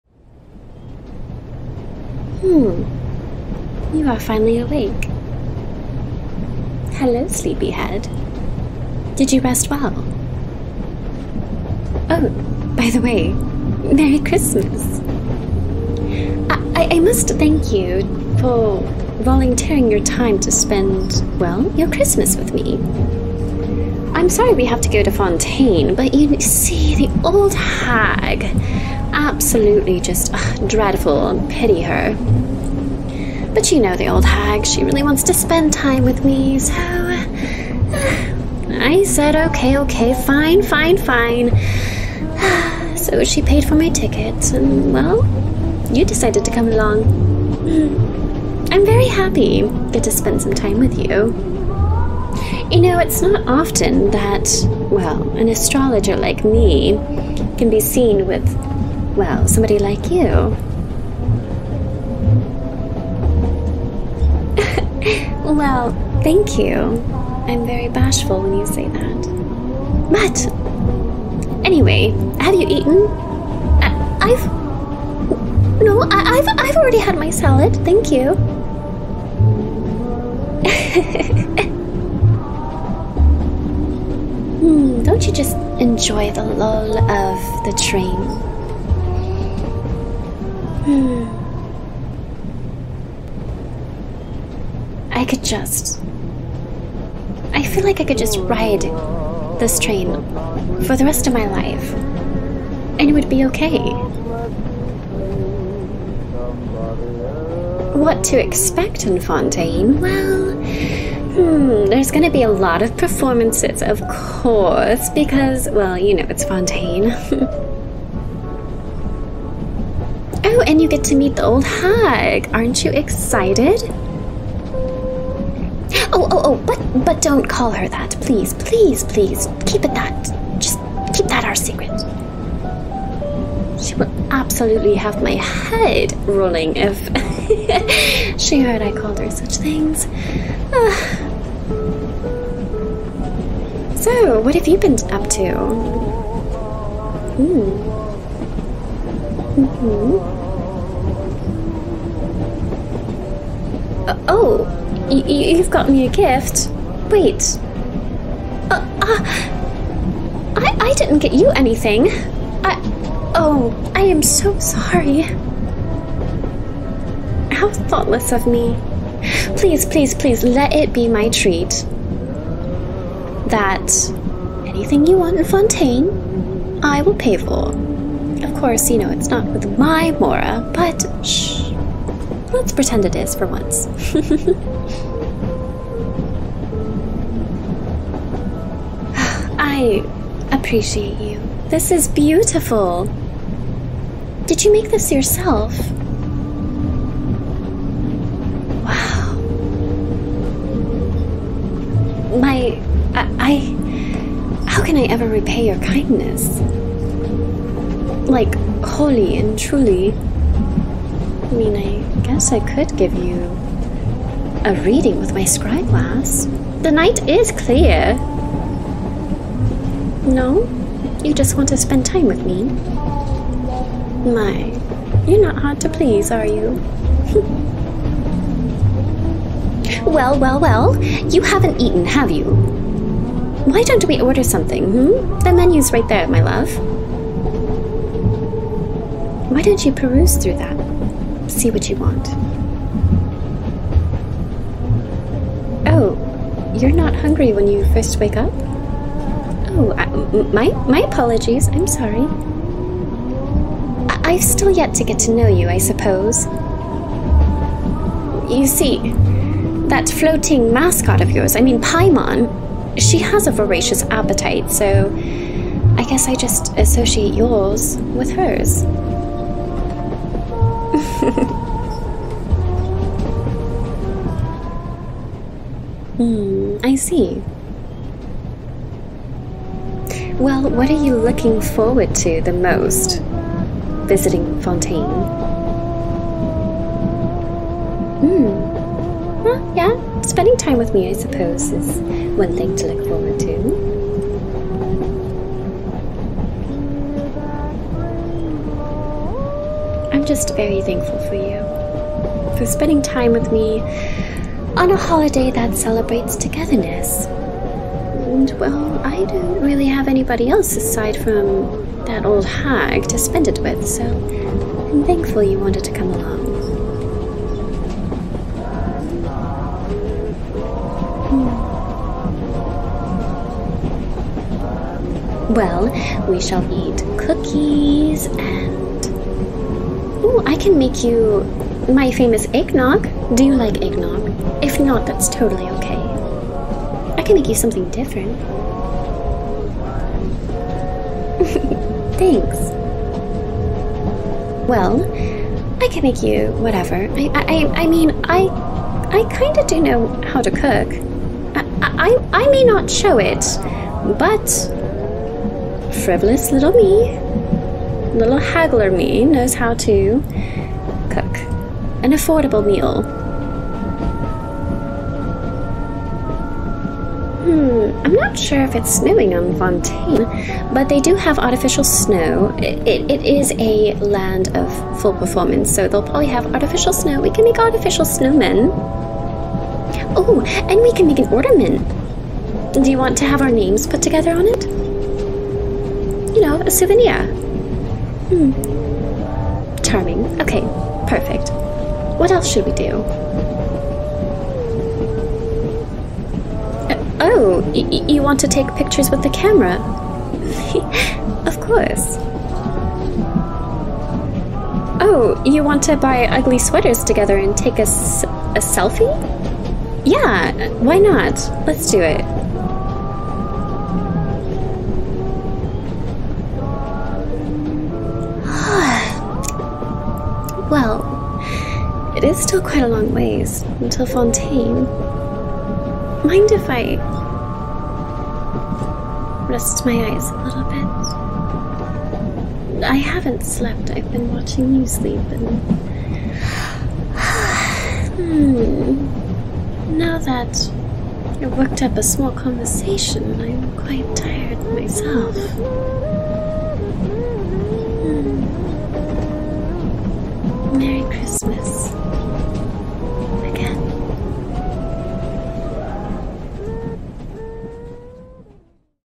[F4A] Christmas with Mona ~ [ASMR | Mona x Listener]
You wake up finding yourself still on the train, with a beautiful astrologer staring at you ~ You are on your way to spend Christmas with her!

First off, thank you @SeepingBl00dd for creating a Christmas Mona art for this video! It would not have been possible for me to do this video without your help. Please, snowcubs, be sure to thank him for his hard work and kindness on this art!

Secondly, my Mona is British sounding, so while I'm still trying to hone in on her accent, this is the best I can manage as I still slip into a British accent every time I do Mona impressions.

Thirdly, oldies on the train audio is by Dreamscapes and this episode can be found on Spotify!